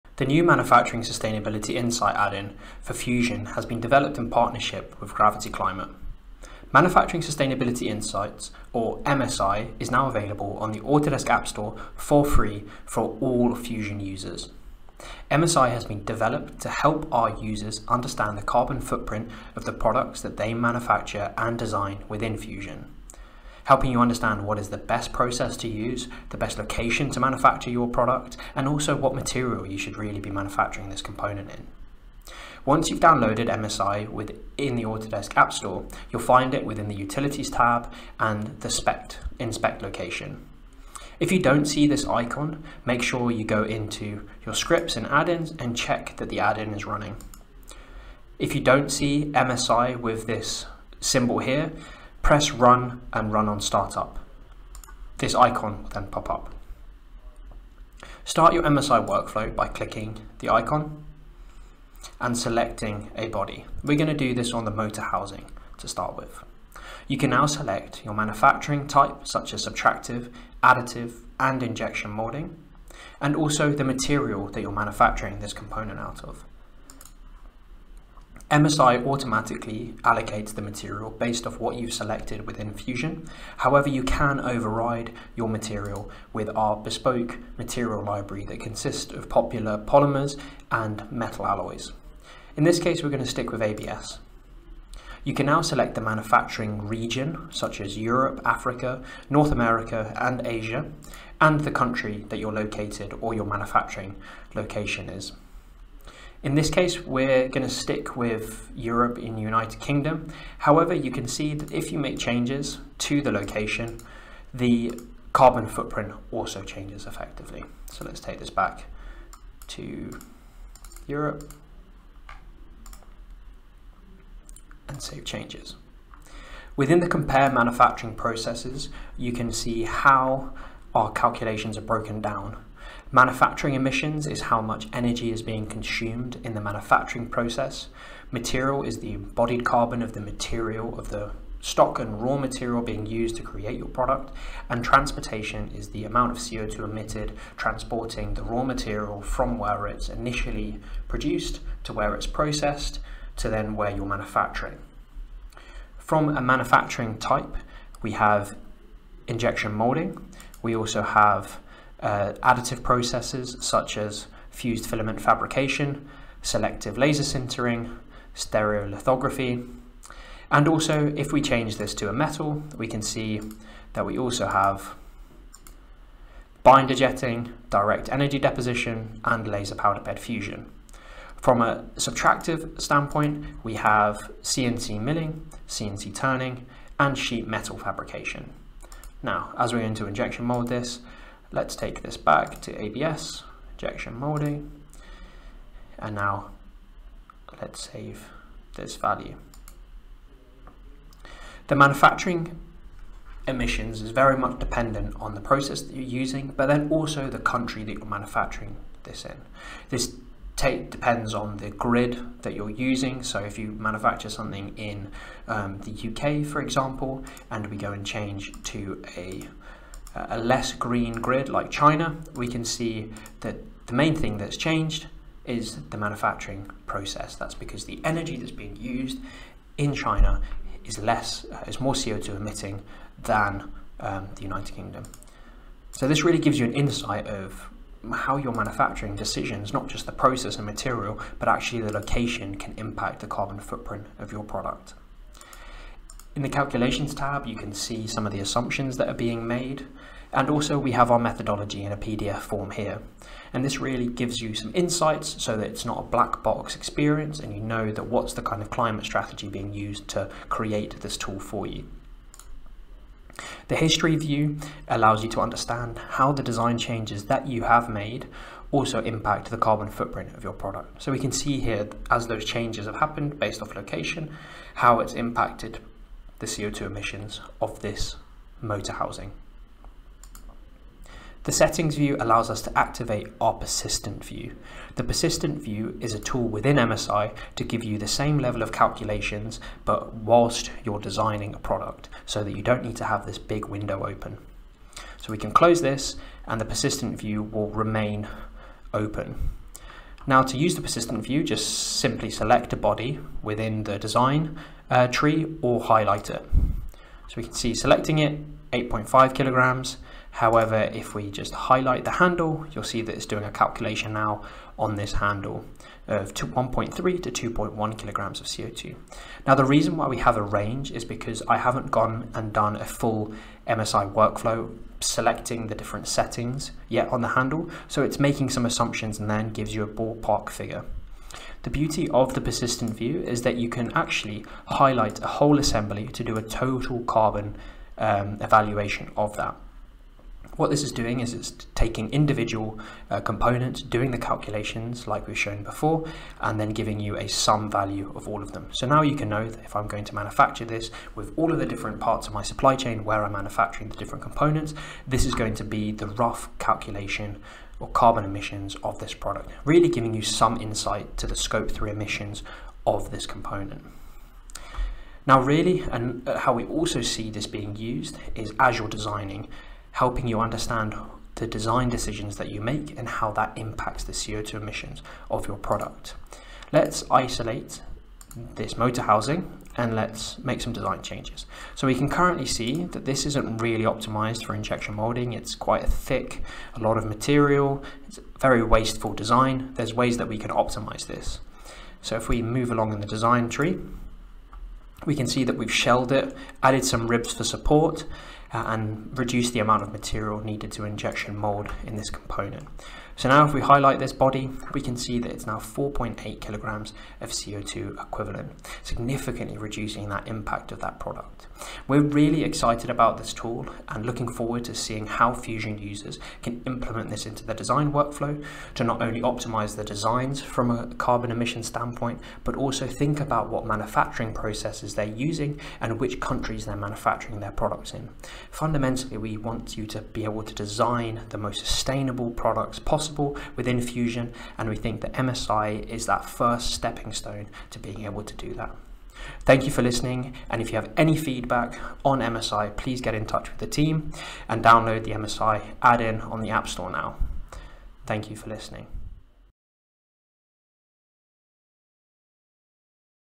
Get Manufacturing Sustainability Insights for Autodesk Fusion
With this new Autodesk Fusion add-on, Manufacturing Sustainability Insights (MSI), we're tackling carbon at the design stage, which contributes to up to 80% of a product's lifetime emissions. This add-on gives you an unprecedented, real-time glimpse into the carbon impact of your design and manufacturing decisions. Whether it's material selection, manufacturing process, or geographical factors, MSI equips Fusion users with critical information at a glance.

With the simple click of a button, you can calculate the carbon emissions of your designs and explore alternative solutions to lower your product's carbon footprint.